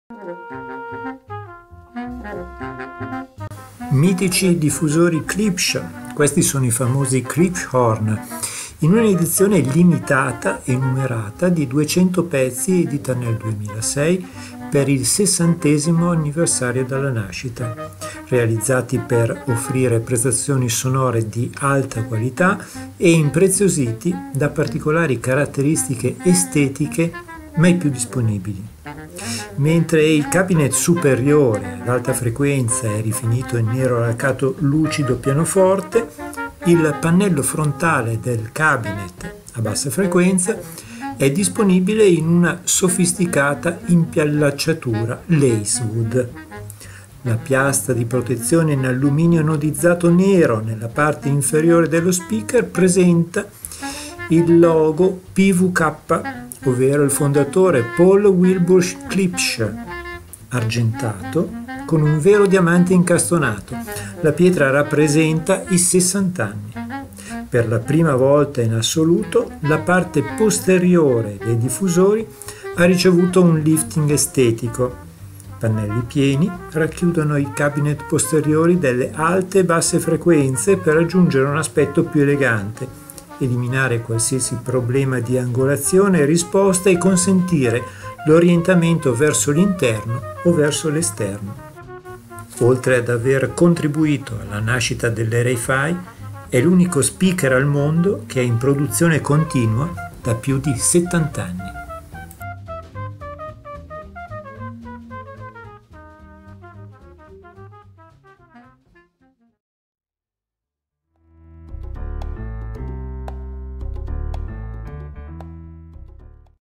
Klipsch Klipshorn 60th Anniversary
Questi diffusori Klipsch - Klipschorn 60th Anniversary sono disponibili presso il negozio di Audio Graffiti a Pandino CR Italy - Vedi questo

Mitici diffusori Klipsch, questi sono i famosi Klipschorn, in una edizione
limitata e numerata di 200 pezzi edita nel 2006 per il sessantesimo
anniversario dalla nascita, realizzati per offrire prestazioni sonore di
alta qualità e impreziositi da particolari caratteristiche estetiche mai
più disponibili.
Mentre il cabinet ad alta frequenza di ogni speaker è rifinito in nero
laccato lucido pianoforte, il pannello frontale del cabinet a bassa
frequenza è disponibile in una sofisticata impiallacciatura Lacewood. La
piastra di protezione in alluminio anodizzato nero nella parte inferiore
dello speaker presenta il logo PWK (il fondatore Paul Wilbur Klipsch)
argentato con un vero diamante incastonato, la pietra rappresenta i 60
anni.
Per la prima volta in assoluto, la parte posteriore dei diffusori ha
ricevuto un "lifting" estetico. Pannelli pieni racchiudono i cabinet
posteriori delle alte e basse frequenze per aggiungere un aspetto più
elegante, eliminare qualsiasi problema di angolazione e risposta e
consentire l'orientamento verso l'interno o verso l'esterno.
Sul retro di ogni Klipschorn anniversario sono presenti anche due
finestre espositive per mostrare la rete crossover aggiornata e una
targa argentata incisa che rende omaggio a P.W.K. e riporta il numero
di produzione del diffusore.
Per la purezza del segnale e la perfetta integrazione del driver, i
crossover sono realizzati con componenti di prima qualità e sono dotati
di cablaggio interno Van den Hul di alta qualità. I supporti della griglia
magnetica consentono un fissaggio sicuro e una facile rimozione delle
griglie relative alle basse frequenze, mentre i connettori di
collegamento WBT di prima qualità assicurano una connessione sicura
con qualsiasi tipo di cavo.
"Sebbene il Klipschorn sia in circolazione fin dai primi giorni dell'Hi-Fi,
è ancora uno degli speaker più performanti sul mercato", ha affermato
Mark Kauffman, Senior Product Specialist. "Oltre ad aver contribuito
alla nascita dell'era Hi-Fi, è l'unico speaker al mondo che è in
produzione continua da più di 70 anni".
Storia di Klipschorn
Insoddisfatto della qualità del suono dei fonografi e dei primi sistemi di
altoparlanti, Paul W. Klipsch utilizzò principi scientifici per sviluppare un
altoparlante "corner horn" che suonasse più realistico. Lo chiamò
Klipschorn.
I cinema e le sale concerti avevano da tempo utilizzato le trombe per
riprodurre le colonne sonore dei film, tuttavia quelle trombe, lunghe
più di nove piedi, erano decisamente troppo grandi per essere
utilizzate in casa. La soluzione di PWK fu quella di ripiegare la tromba
su se stessa e posizionare il cabinet dell'altoparlante in un angolo,
rendendo le pareti della stanza un'estensione della tromba.
Un successo indiscutibile, il Klipschorn, brevettato nel 1945,
rappresenta un'eredità che ha guidato l'ingegneria degli altoparlanti
Klipsch nel corso degli anni. Ogni altoparlante Klipsch è progettato
secondo la stessa filosofia del suono dal vivo che ha ispirato il
Klipschorn.
Gli speakers sono in ottime condizioni senza alcun segno.
Disponibili anche le griglie in tessuto laterali non visibili in foto.

CARATTERISTICHE TECNICHE:
RISPOSTA IN FREQUENZA (+/- 4 dB) 33 Hz – 17 kHz
SENSIBILITÀ 105 dB a 2,83 V / 1 m
GESTIONE DELLA POTENZA (CONT/PICCO) 100 W/400 W
SPL MASSIMO 121 dB Continuo
IMPEDENZA NOMINALE 8 ohm Compatibile
FREQUENZA DI CROSSOVER HF: 4500 Hz
MF: 450 Hz
DRIVER PER LE ALTE FREQUENZE: K-77-D 1" (2,54 cm) diaframma
fenolico con driver a compressione
MIDRANGE K-55-X 2" (5,08 cm) diaframma fenolico driver a
compressione
DRIVER PER LE BASSE FREQUENZE K-33-E 15" (38,1 cm) cono in fibra
composita
woofer con un esponenziale triedrico piegato a corno
MATERIALE DEL MOBILE Compensato di betulla e MDF
INGRESSI Doppi morsetti / bi-wire / bi-amp
ALTEZZA (128,9,4 cm)
LARGHEZZA (79,38 cm)
PROFONDITÀ (71,75 cm)